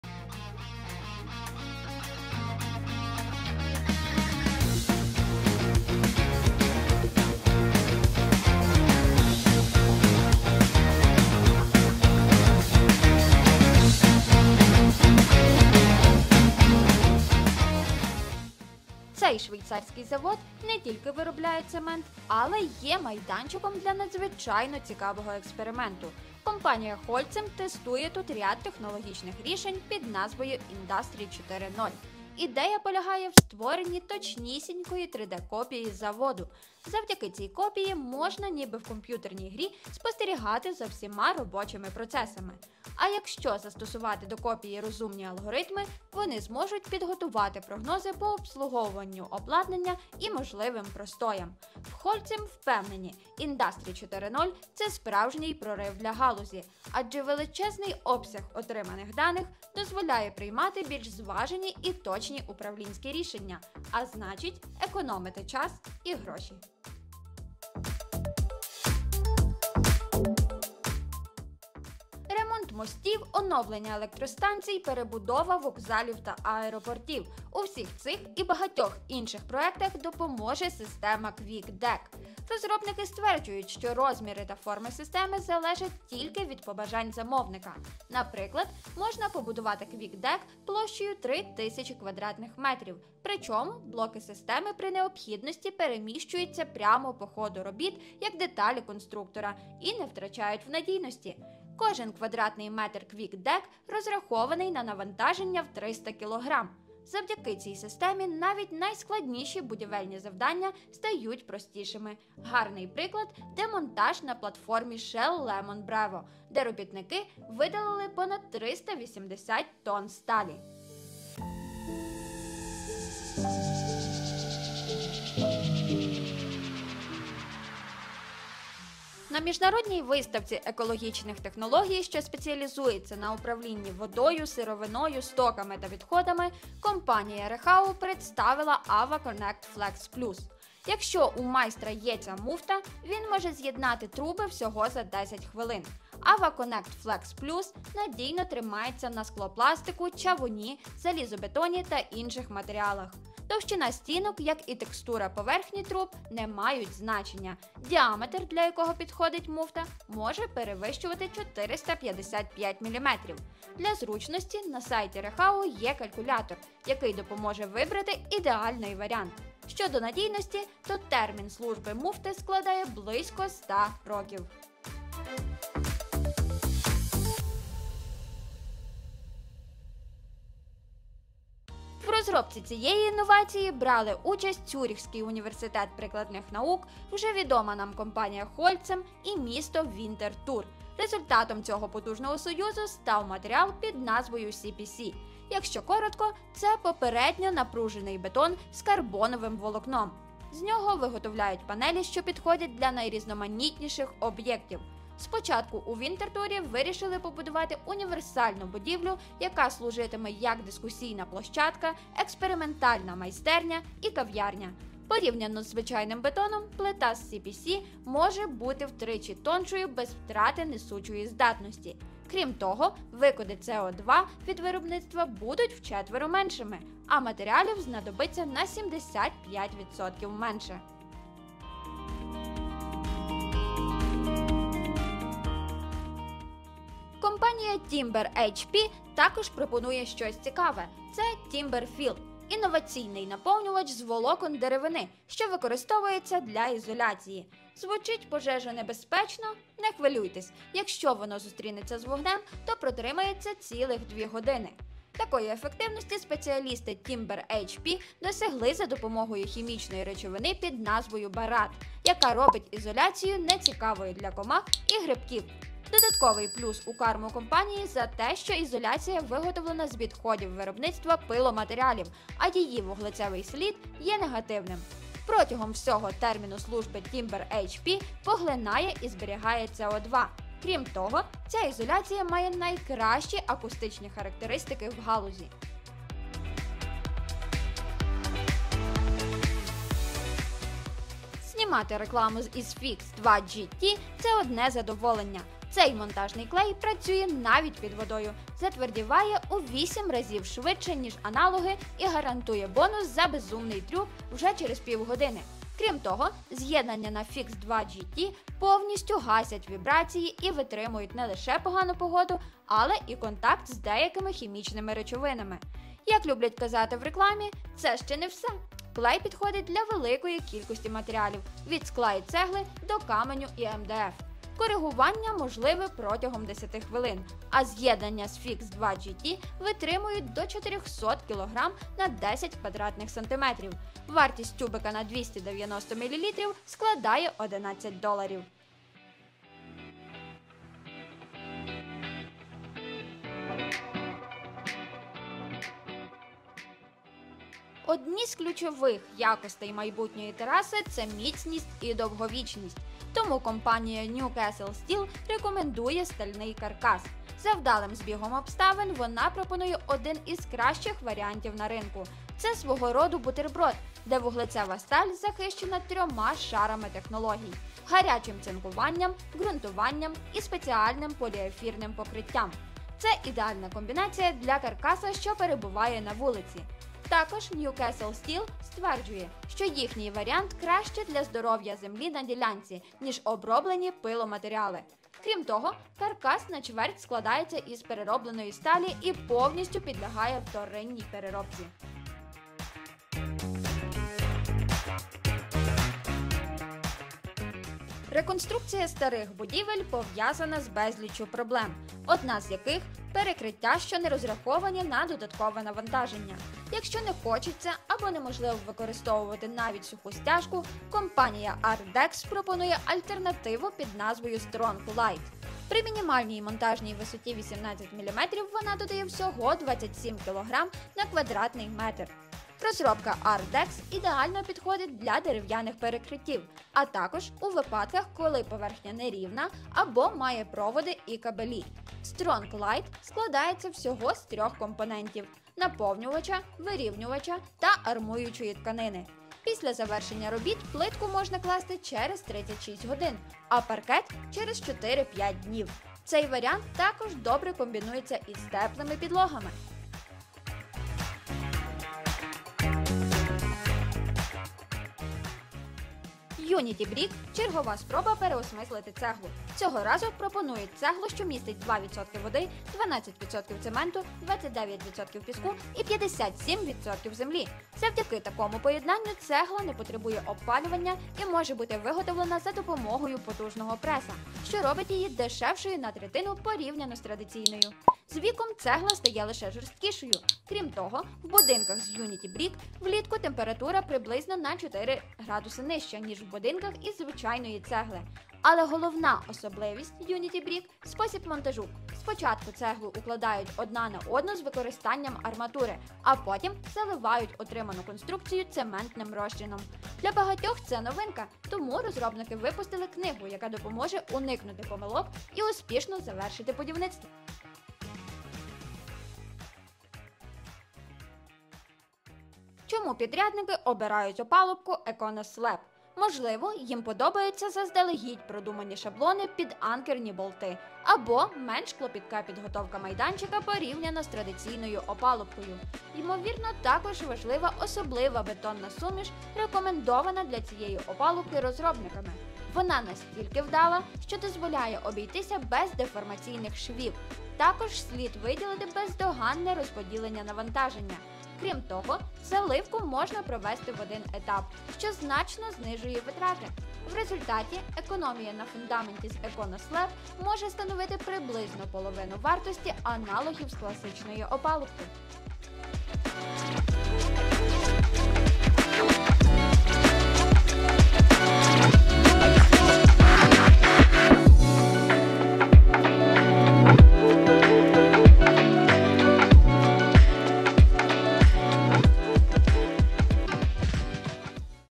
Прогреси будівництва: Як сучасні технології скорочують витрати та час на будівництво у 2 рази!?
Якщо ти читаєш цей опис, то, очевидно, у тебе є гарне відчуття стилю і креативу, яке чекає на реалізацію! Сьогодні ми познайомимо тебе з новими будівельними технологіями, які допоможуть втілити твої божевільні ідеї у життя і зробити твій проект незабутнім!
Почнемо з технології Holcim Cement Plant Digital Twin — це дійсно щось фантастичне. Уяви собі: цифрова копія будівлі, яка дозволяє в реальному часі спостерігати за всіма будівельними процесами. Наче у комп'ютерній грі, де ти бачиш кожен механізм і деталь, але тут все серйозніше — технологія не тільки показує, що відбувається, але й допомагає прогнозувати можливі поломки чи простої. Це революційний прорив, який дає змогу економити величезні ресурси — час, гроші та технічні зусилля.
Також не можна пропустити REHAU AWACONNECT Flex Plus — надзвичайно зручну муфту, яка дозволяє швидко та надійно з'єднувати труби. Час на з'єднання — лише 10 хвилин, і не має значення, з яких матеріалів зроблені труби. Ця муфта працює і з чавуном, і з залізобетоном, і навіть зі склопластиком. Її строк служби — до 100 років, тому це ідеальне рішення для довговічних інфраструктурних проектів.
Наступне — Holcim CPC Panels. Це плити з попередньо напруженого бетону з карбоновим волокном, які втричі тонші за звичайні бетонні плити, але не поступаються в міцності. Крім того, їх виробництво скорочує викиди CO2 в чотири рази! Це чудове рішення для тих, хто прагне будувати екологічно та економно, адже використання матеріалів скорочується на 75%.
І нарешті, ми не можемо не згадати про TimberHP TimberFill — інноваційний матеріал для теплоізоляції, який виготовляється з відходів деревини. Крім того, що він має відмінні акустичні властивості, TimberFill також вогнестійкий і може витримати до двох годин прямого контакту з вогнем! Це рішення не тільки екологічне, але й абсолютно безпечне, оскільки не приваблює комах та грибки.
Ось тобі і щось простіше! Монтажний клей, який справді здивує вас своїми можливостями. Він працює навіть під водою і висихає у вісім разів швидше за аналоги.  Такий клей стане незамінним для складних будівельних проектів, де потрібна максимальна надійність.
Не можна обійти стороною Advantage Lumber New Castle Steel Deck Framing — ідеальний варіант для міцних і довговічних терас. Стальний каркас, захищений трьома шарами технологій, гарантує стійкість до зовнішніх факторів та повністю підлягає вторинній переробці.
Далі звернемо увагу на ARDEX STRONGLITE — інноваційне рішення для реконструкції старих будівель. Ця легка стяжка створена спеціально для перекриттів, що не розраховані на великі навантаження. Вона чудово підходить для дерев’яних перекриттів та важкодоступних місць, зберігаючи низьку вагу і забезпечуючи швидкий монтаж.
Не менш цікавою є Uniti Brick — новий підхід до виготовлення цегли. Вона на третину дешевша за традиційну та стає міцнішою з часом. Спеціальний метод монтажу з використанням арматури та цементного розчину робить її відмінним рішенням для сучасного екологічного будівництва. Цегла з Unity Brick допомагає зберігати прохолоду в будинку влітку і є економічною альтернативою звичайним матеріалам.
І нарешті, поговоримо про Econo-Slab — ефективну систему опалубки для фундаментів. Вона дозволяє зменшити витрати на фундамент до половини, завдяки спеціально продуманим шаблонам і унікальній бетонній суміші. Це значно прискорює процес будівництва і знижує витрати на підготовку майданчика.
Отож, як бачиш, світ будівельних технологій постійно змінюється, і ці інновації не просто полегшують життя будівельникам, а й роблять майбутнє більш стійким і розумним.
Зміст:
00:18 3D в будівництві: як попередити негаразди у будівництві
01:12 Система для будівництва електростанцій, мостів, вокзалів та аеропортів
02:10 Будівельні труби, як правильно їх з’єднати
03:01 Бетонні панелі, що пришвидшують процес будівництва
04:00 Теплоізоляція у будинку, стійка до вогню теплоізоляція
04:50 Купити монтажний клей миттєвої дії
05:48 Як зробити терасу своїми руками, сучасні тераси у будинку?
06:40 Реконструкція будинку, як зробити перекриття міцнішим?
07:37 Інноваційна цегла, цегла повнотіла, цегла пустотіла, найкраща цегла для будівництва
08:35 Опалубка для фундаменту, заливка фундаменту в один етап